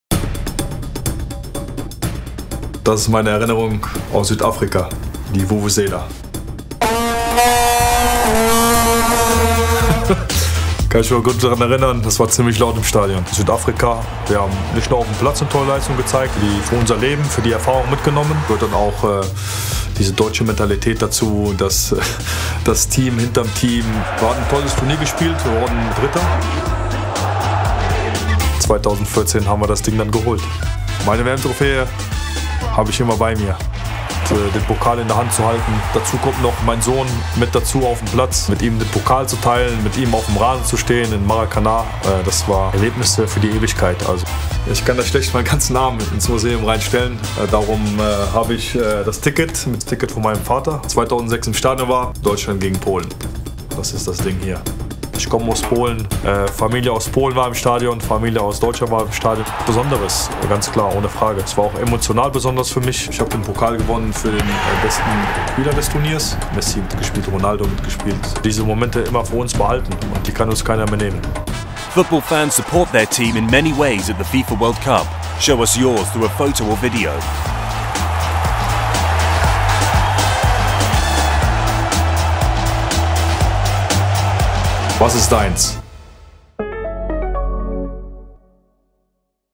[Hyundai Story] 2018 FIFA 러시아 월드컵 | 현대 월드 풋볼 헤리티지 with 포돌스키
당신은 어떻게 국가대표팀을 응원하시나요?

독일 축구스타 포돌스키는 관중들의 열정적인 응원이 경기의 모든 것을 바꿔놓는다고 말합니다.

지금 현대자동차 월드컵 홈페이지를 방문해, 응원의 중요성을 확인해보세요.